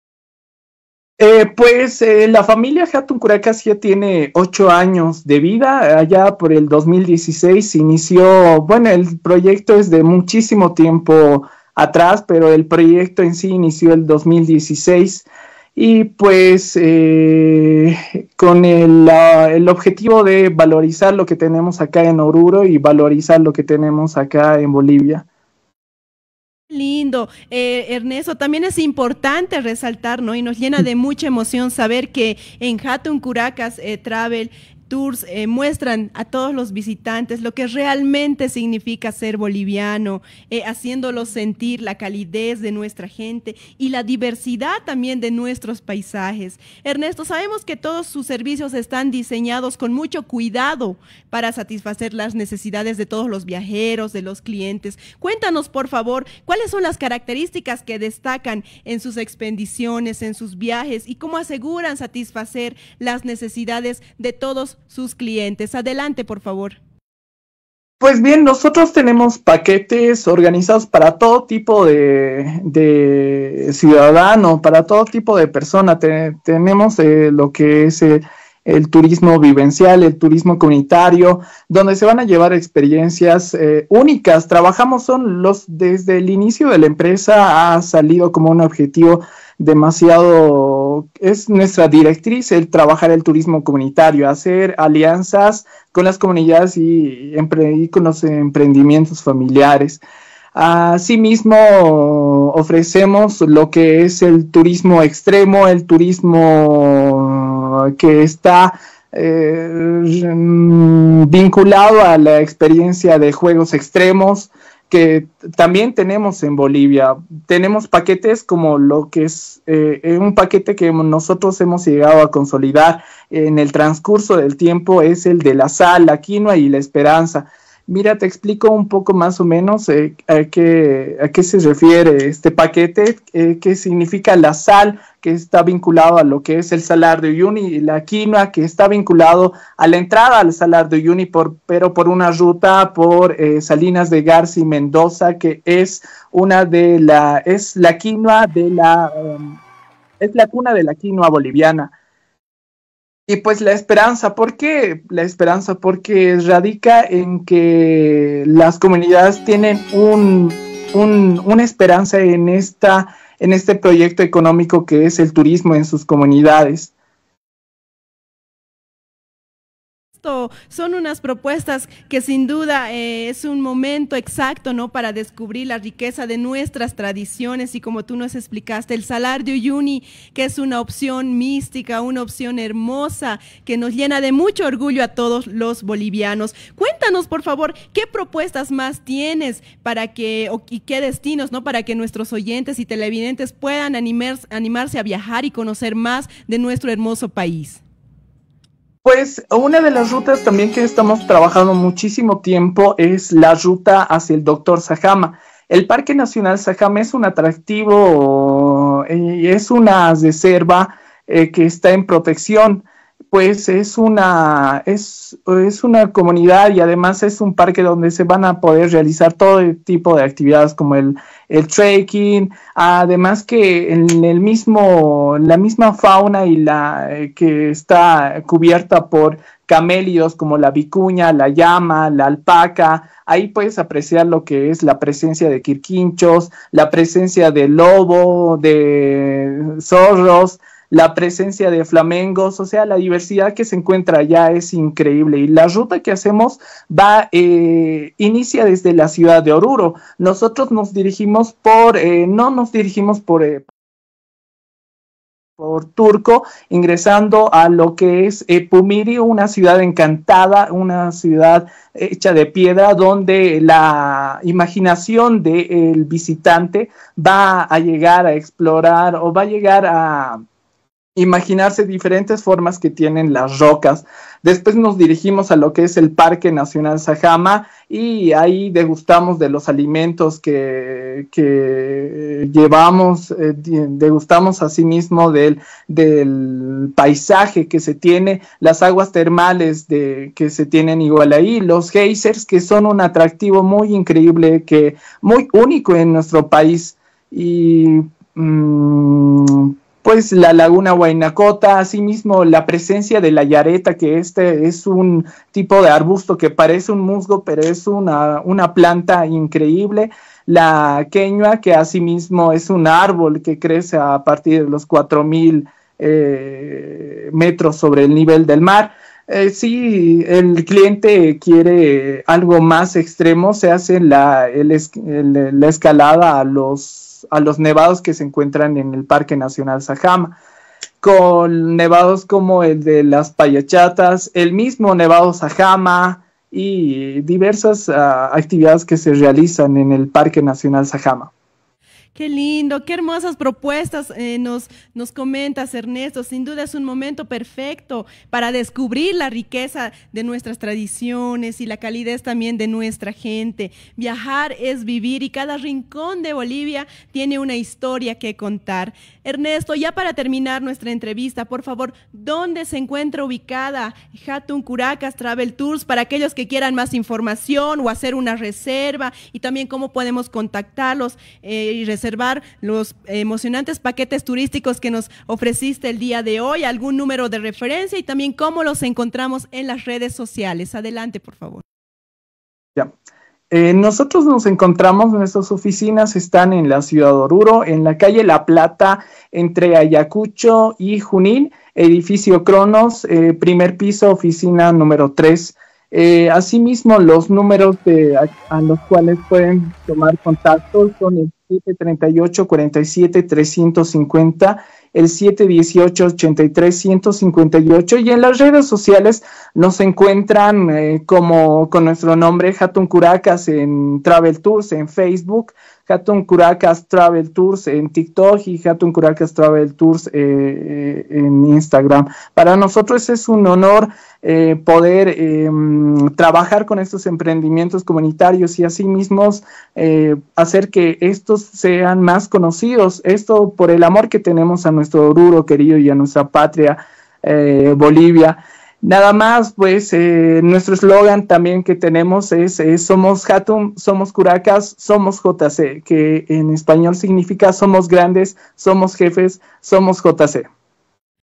🌍✈️ ¡Conoce Bolivia con Jhatun Curacas Travel Tours! 🏞️✨
¿Estás buscando una experiencia única? ¡Jhatun Curacas es la agencia de viajes que te lleva más allá de lo común! Ofrece turismo vivencial, comunitario y de aventura, para que vivas las culturas, tradiciones y paisajes de forma auténtica y emocionante.
📞 Contáctalos y empieza tu aventura: 73847350